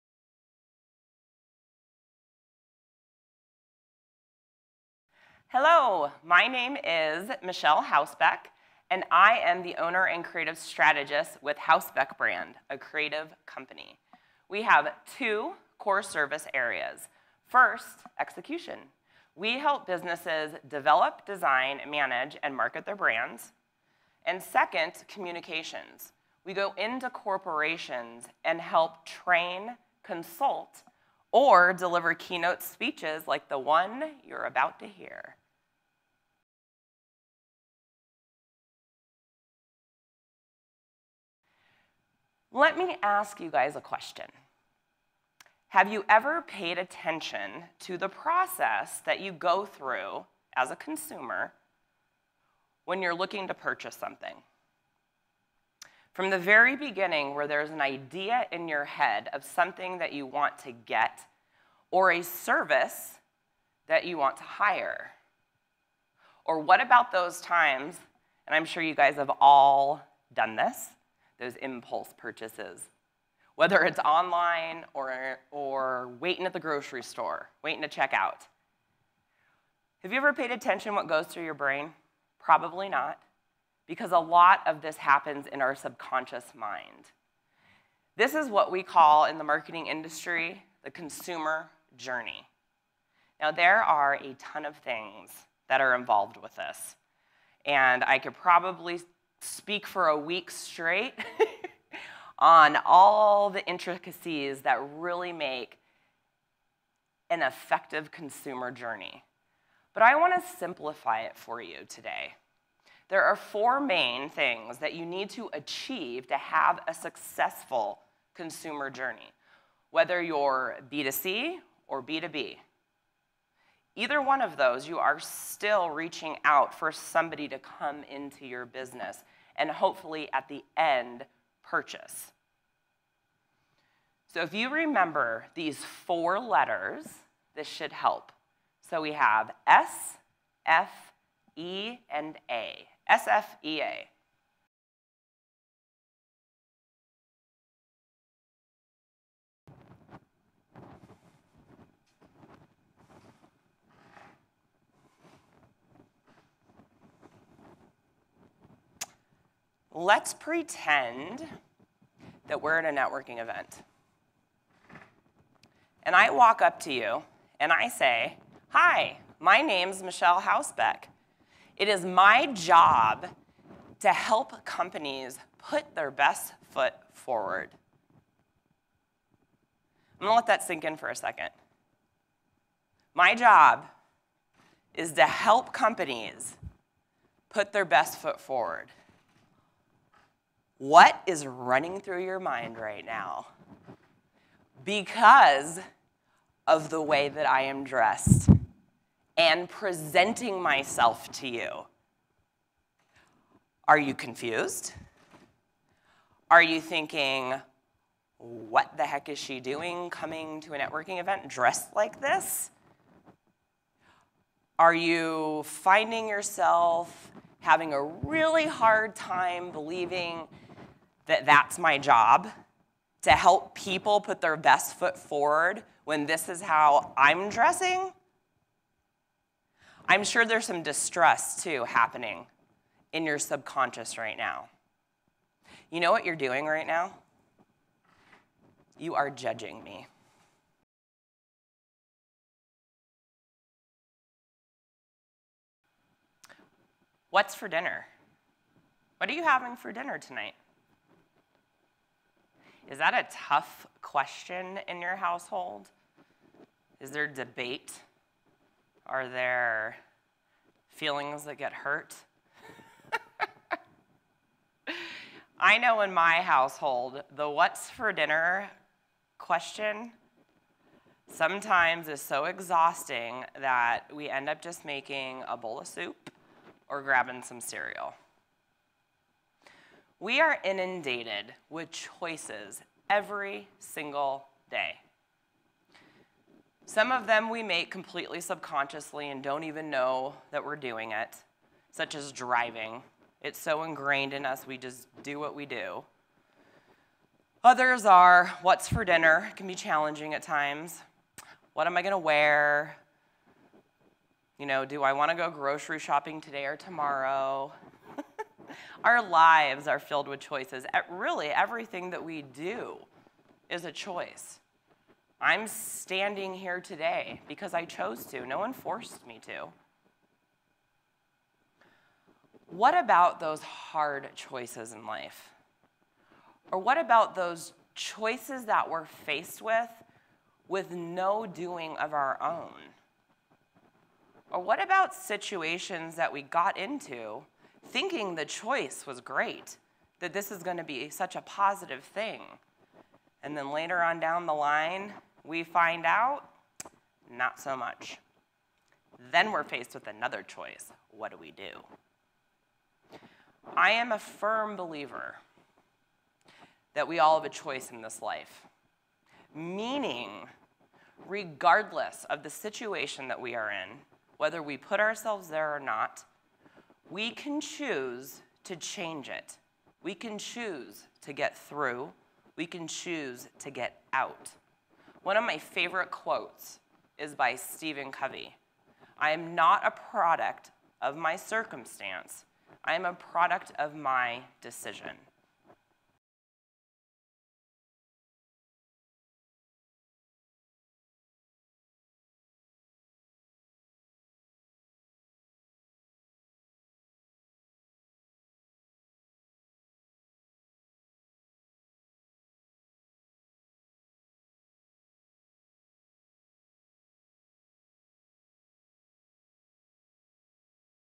Michelle Hausbeck Speaker Reel
Michelle is a frequent keynote speaker for both personal and industry-specific topics.

She has worked in the design and marketing industry since 2000 and has had a rollercoaster of a ride in her life so far. She has learned that personal and professional lives always overlap. The choices we make in our business lives affect our personal lives and vice versa. Her mission is to utilize her skills, life experiences and lessons learned to assist her clients in turning their passions into reality so their next chapters can be designed on their terms!

Michelle believes we all deserve to live our best life. We deserve to do something each and every day that we are passionate about. We deserve to be proud of what we do. We deserve joy!

"I am not a product of my circumstances, I am a product of my decisions." – Stephen Covey